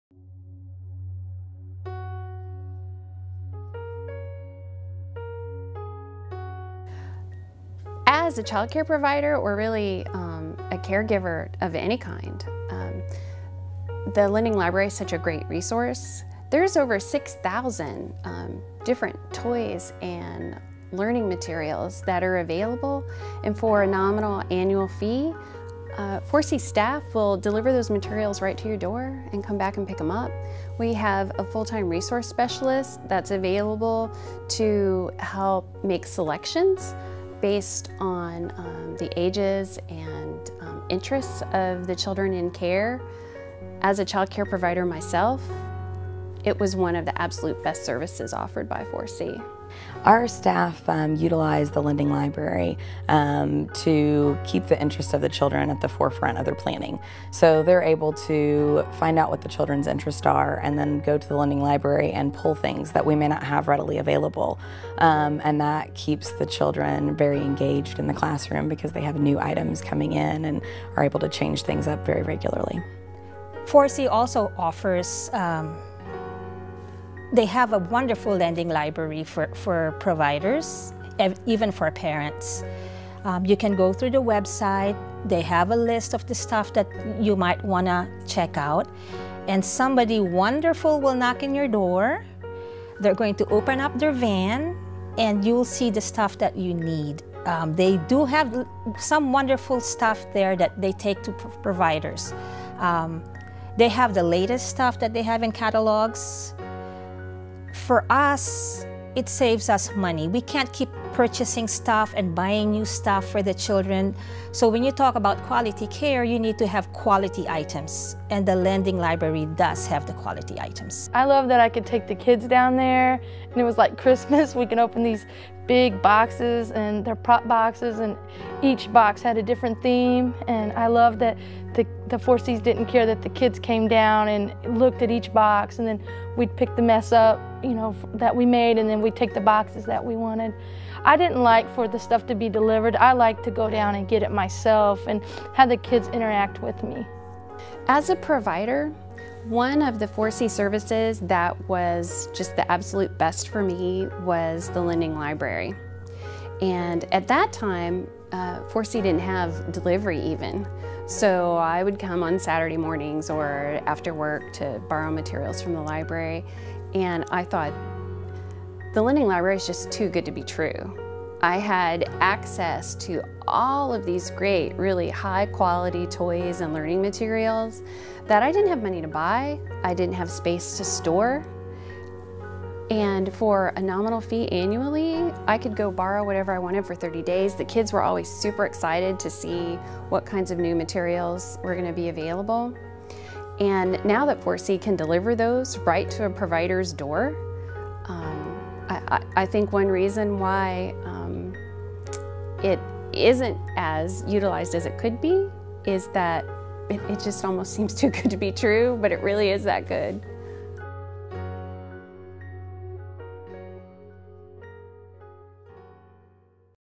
4C Lending Library
Child care providers and 4C's CEO describe the benefits of the 4C Lending Library and why it's important to them.